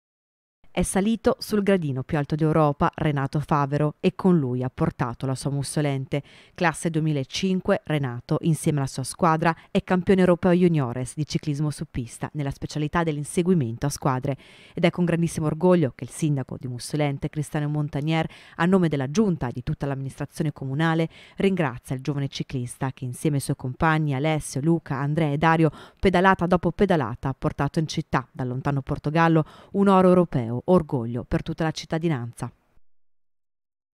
ORGOGLIO IN CITTA': RENATO FAVERO E' CAMPIONE EUROPEO DI CICLISMO SU PISTA | 17/07/2022
TG Bassano del Grappa 17/07/2022 - MUSSOLENTE - Renato Favero, classe 2005 di Mussolente, insieme alla sua squadra, è campione europeo juniores di ciclismo su pista.  || Ed è salito sul gradino più alto d'Europa, Renato Favero, e con lui ha portato la sua Mussolente. Classe 2005, Renato, insieme alla sua squadra, è campione europeo juniores di ciclismo su pista, nella specialità dell'inseguimento a squadre. Ed è con gradissimo orgoglio che il Sindaco Cristiano Montagner, a nome della giunta e di tutta l'amministrazione comunale, ringrazia il giovane ciclista, che insieme ai suoi compagni, Alesssio, Luca, Andrea e Dario, pedalata dopo pedalata, ha portato in città, dal lontano Portogallo, un oro europero, orgoglio per tutta la cittadinanza. (Servizio di Sharon Di Carlo) - Segui Rete Veneta anche sul digitale terrestre! Visita il sito